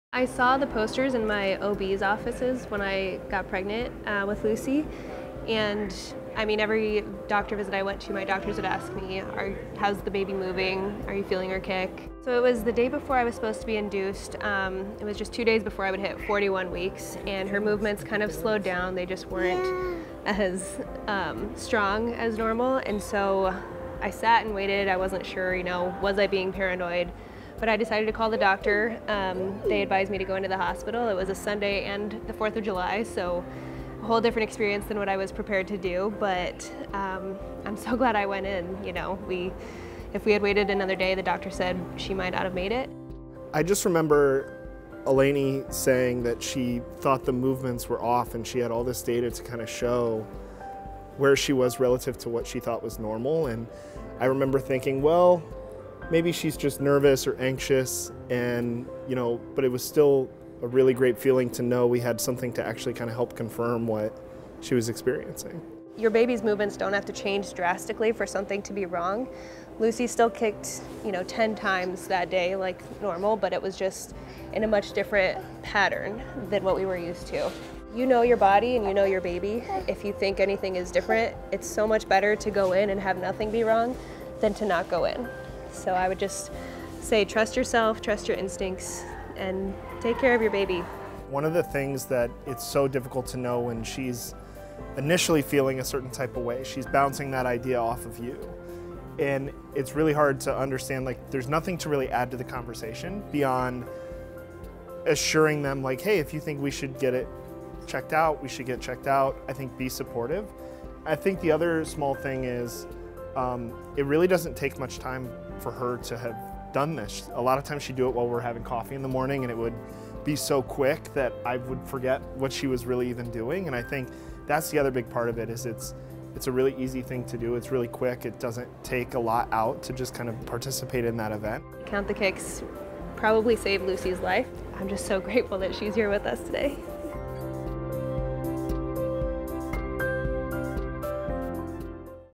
Lucy's Baby Save Story
Lucy’s parents Eleni and Adam share their experience with the Count the Kicks app and explain how it led to Lucy’s safe arrival. Third trimester? Get to know your baby's movements using the free Count the Kicks app.